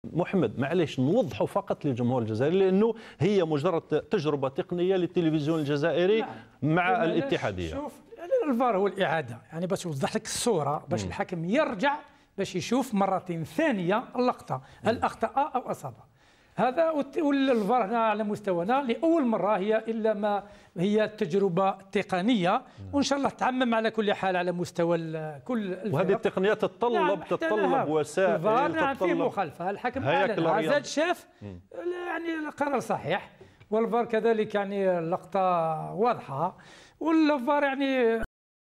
VAR Première utilisation en Algérie
L'Algérie  tester le VAR cette semaine lors des rencontres MC Alger-USM Bel Abbès